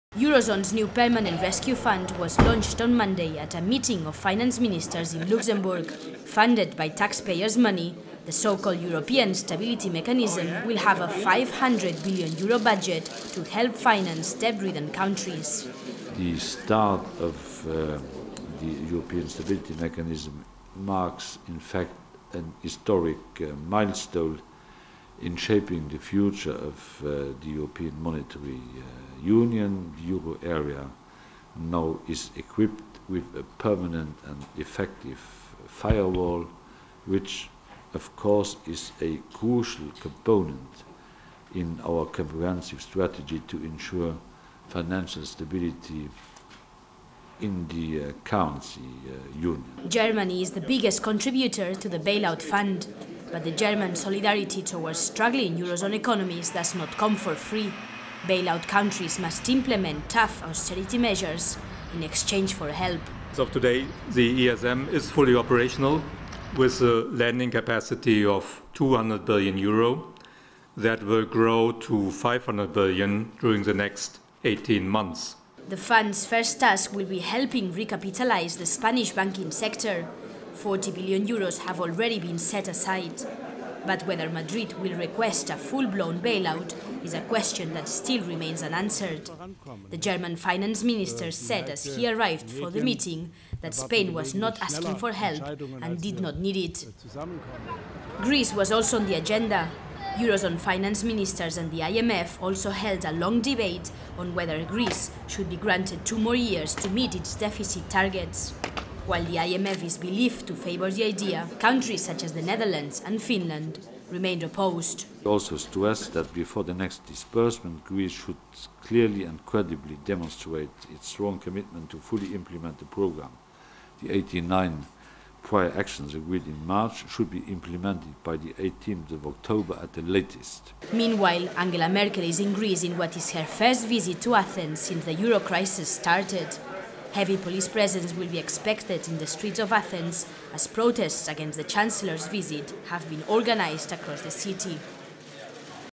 Eurogroup launches permanent rescue fund, says Spain does not need bailout
Eurozone's new permanent rescue fund was launched on Monday at a meeting of finance ministers in Luxembourg. Funded by taxpayers' money, the so-called European Stability Mechanism will have a 500 bn euro budget to help finance debt-ridden countries.

The start of the European Stability Mechanism marks an historic milestone in shaping the future of the European monetary Union. The Euro area now is equipped with the effective firewall which of course is a crucial component of our strategy to ensure financial stability in the Union', said president of the Eurogroup Jean-Claude Juncker.

Germany is the biggest contributor to the bailout fund. But the German solidarity towards struggling eurozone economies does not come for free. Bailed out coutries must implement tough austerity measures and reforms in exchange for help.

'As of today the ESM is fully operational with lending capacity of 200 billion euro that will grow in the next 18 months. In addition to that we will have the 92 billion euro of the EFSF which is committed for the macroeconomic adjustment program of Ireland, Portugal and Greece', said Managing Director of the ESM Klaus Regling.

The fund's first task will be helping recapitalise the Spanish banking sector. 40 bn euros have already been agreed.

Whether Madrid will request a full-blown bailout is a question that still remains unanswered. German Finance Minister Wolfgang Schäuble said as he arrived for the meeting that Spain was not asking for help and did not need it.

Greece was also on the agenda.

Eurozone finance ministers and the International Monetary Fund also held a long debate on whether Greece should be granted 2 more years to meet its deficit target. While the IMF is believed to favour the idea, countries such as the Netherlands and Finland remain opposed.

'We also stress that before the next disbursement; Greece should clearly and credibly demonstrate its strong commitment to fully implement the program. The 89 prior actins agreed in March implemented by the 18 of October at the latest', said Juncker.

'Greece the review of the program continues and it is making progress. I believe that there is the necessary willingness on the part of the Greek authorities to agree the measures needed to set Greek public finances back on a sustainable path', said Olli Rehn.

Meanwhile, Angela Merkel is in Greece in what is her first visit to the country since the euro crisis started. Heavy police presence will be expected across Athens as protests against the Chancellor's visit have been organised.

-0-